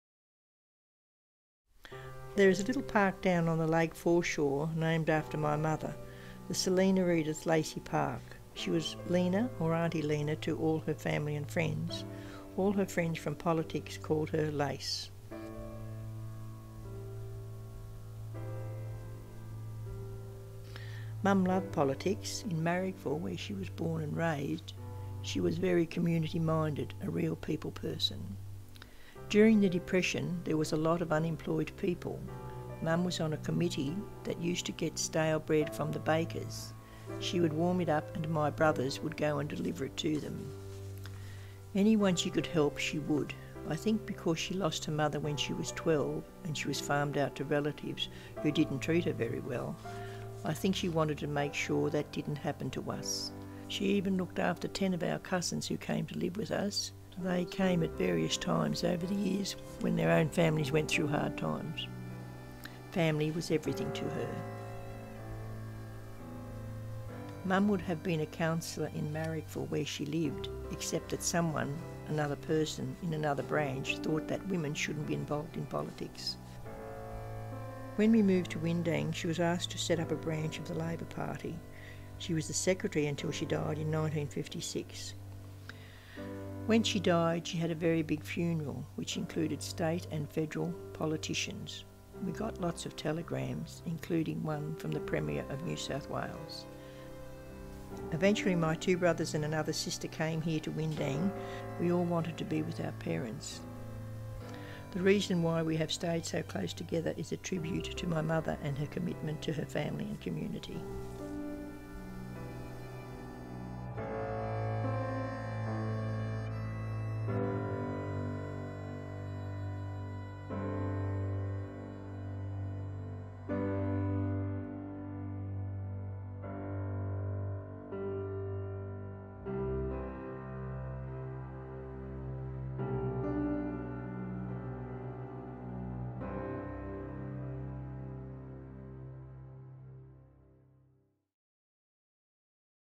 Lace - by Pat Hesse (a digital story from the Lake Illawarra MAP Project)
MENTOR WRITER: Susan Kennedy
SYNOPSIS: Edith was a woman of practical politics, for the sake of her family
SCRIPT: There is a tiny little park down on the lake foreshore named after my mother  the Selina Edith Lacey Park.  She was Lena or Aunty Lena to all her family and friends.  All her friends from politics called her Lace.

Mum loved politics.  She was very community minded, a real people person. There were a lot of unemployed people back then and the bakers used to give them stale bread.  Mum would warm it up and my brothers would go and deliver it to them.

Anyone she could help, she would.  I think because she lost her mother when she was 12 and she was farmed out to relatives who didnt treat her very well.  I think she wanted to make sure that didnt happen to us.   She even looked after 10 or our cousins who came to lives with us in hard times for their own families. Family was everything to her.

Mum would have been a councilor in Marrickville where we lived, except that someone  another person,  in another branch thought that women shouldnt be involved in politics.  When we moved to Windang she was asked to set up a branch of the Labor Party.  She was the secretary until she died in 1956.

When she died she had a very big funeral.  There were politicians there.  We got lots of telegrams.  We got them from the Premier and other politicians.

Eventually my two brothers and another sister came here to Windang.  We all wanted to be with our parents.  The reason why we have stayed so close together is tribute to my mother and her commitment to her family and community.